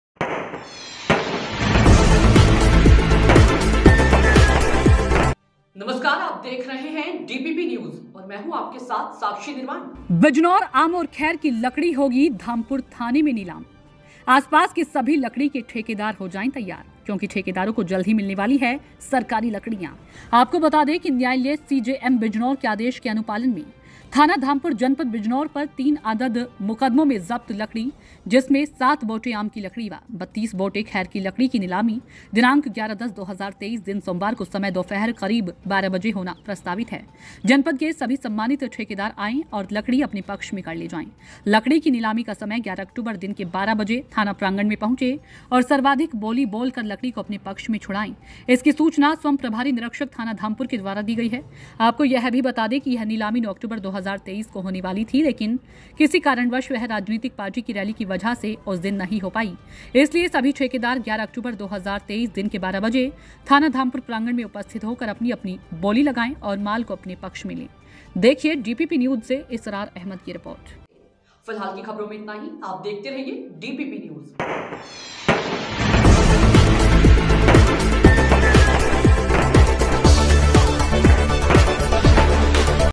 कोतवाली धामपुर में होगी लकड़ियों की नीलामी, आप भी उठा सकते है लाभ | DppNews
आपका सारथी